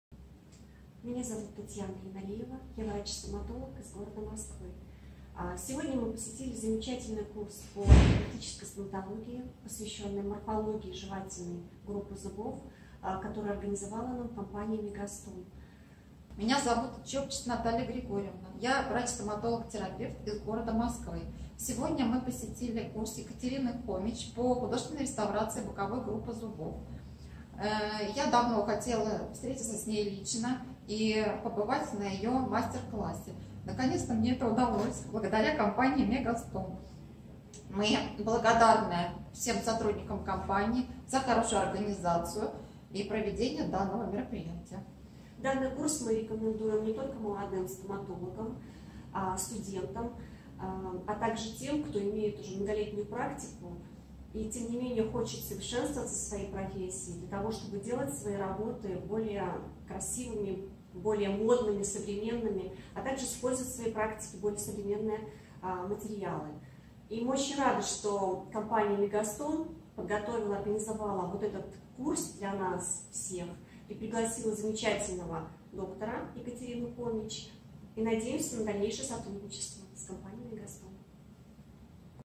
Отзыв о курсе по терапии Екатерины Хомич
Своими впечатлениями о курсе Екатерины Хомич "Анатомия и пропорции идеальной реставрации жевательной группы зубов" поделись Еналиева Татьяна Анатольевна врач-стоматолог и Чопчиц Наталья Григорьевна врач стоматолог-терапевт (г. Москва)

Интересует лечение зубов, пломбирование каналов? Запишитесь на консультацию по телефону в Москве: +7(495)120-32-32